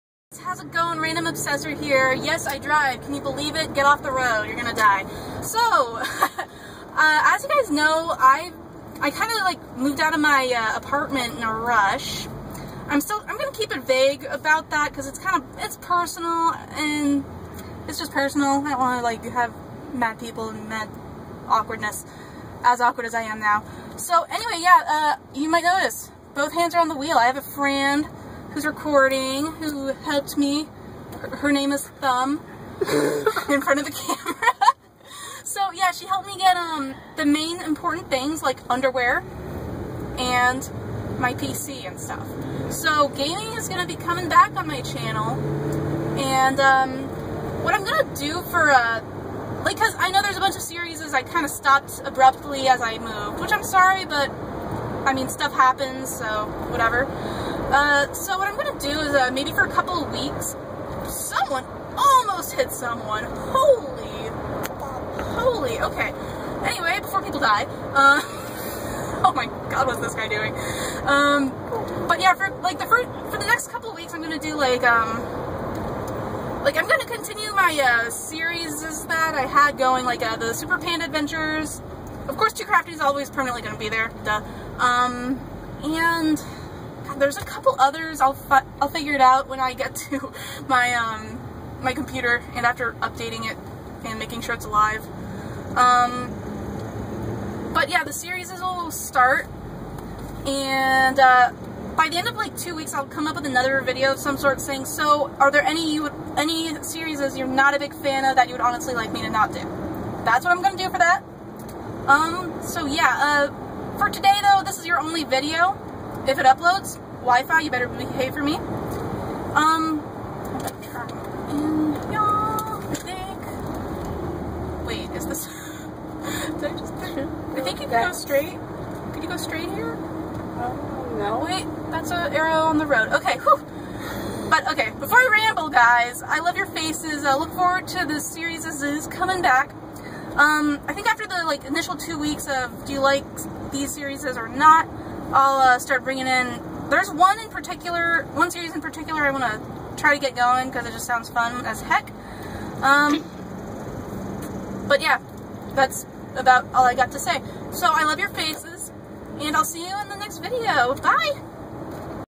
Life Update
I GOT MY STUFF BACK! ALL CAPS NECESSARY!